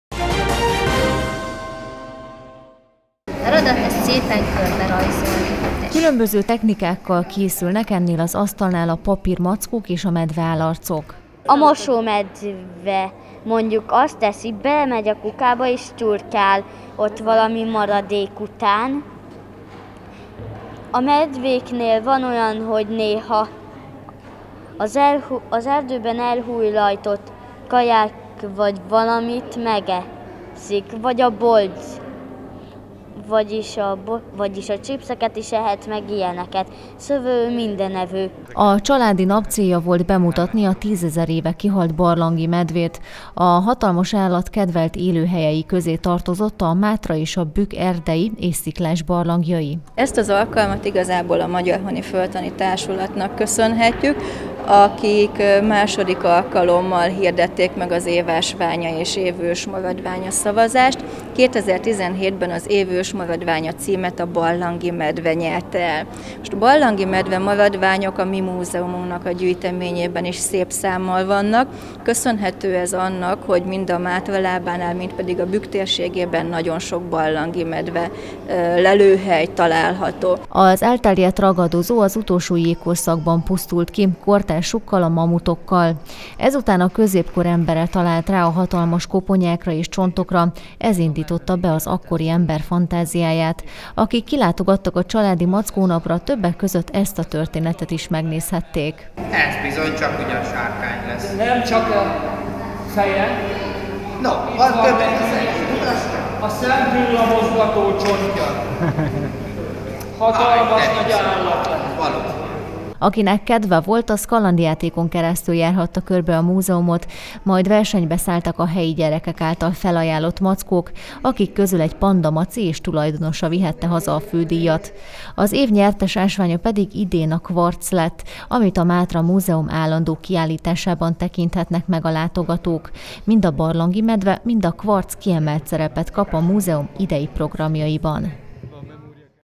Családi Mackó nap az MTM Mátra Múzeumban
2017-ben a Barlangi medve lett az év ősmaradványa. A sokszor a 700 kilót is elérő állatok a Mátra és a Bükk hegységeiben laktak. A barlangi medve kopányájával és sok más érdekességgel is találkozhattak azok akik ellátogattak hétvégén a gyöngyösi Mátra Múzeumba a családi mackó napra.